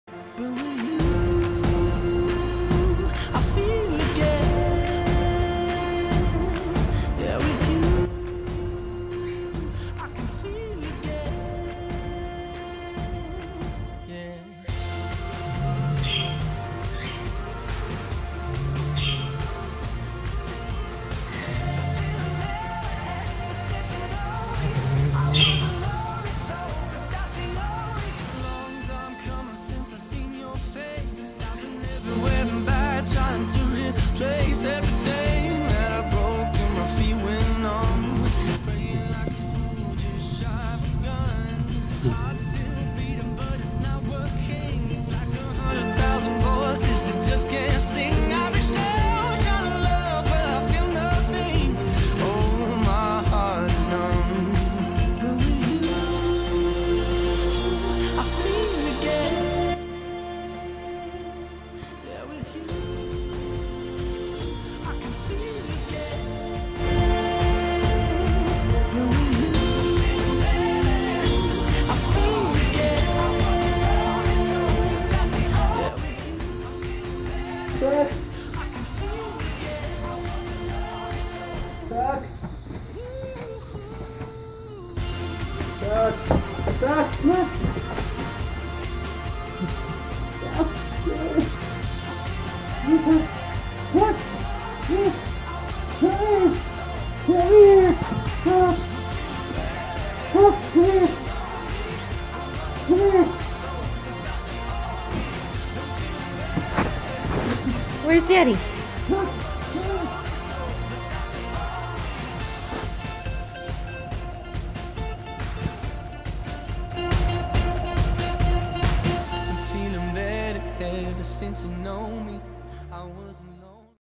Our Moments with Tux
Our moments with Tux, a guide dog in training. We miss him everyday! We'll never forget how this lil pup touched our lives.